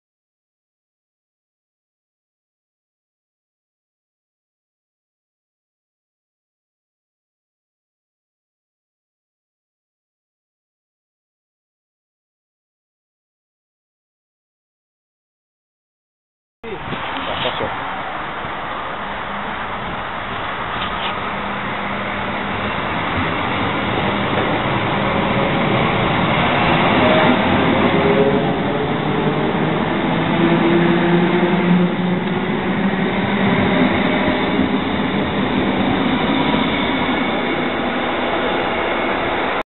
AM 808 Antwerpen-Luchtbal
La 808 quitte Anvers-Luchtbal sur le L 2564 pour Roosendaal (NL).
18-03-15